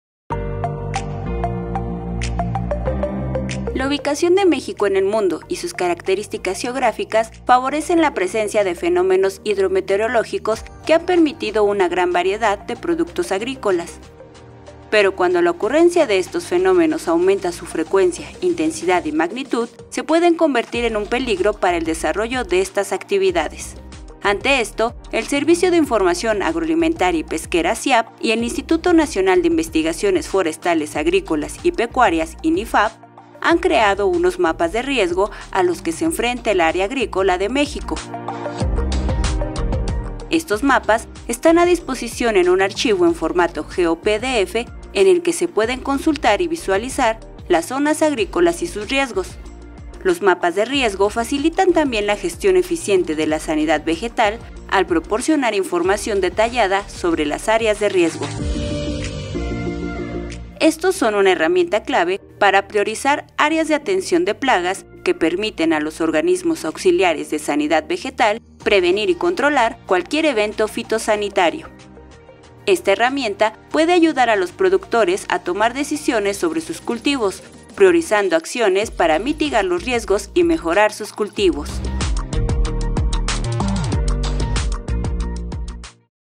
Mapas de riesgo: tecnología que protege al campo mexicano
Los mapas de riesgo son herramientas clave para anticipar y gestionar problemas en el campo, como sequías, plagas o inundaciones. 📉🌦️ En México, estos mapas ayudan a los agricultores a tomar decisiones informadas, optimizando recursos y asegurando mejores cosechas.

Agronoticias, alimentamos tu interés por el campo.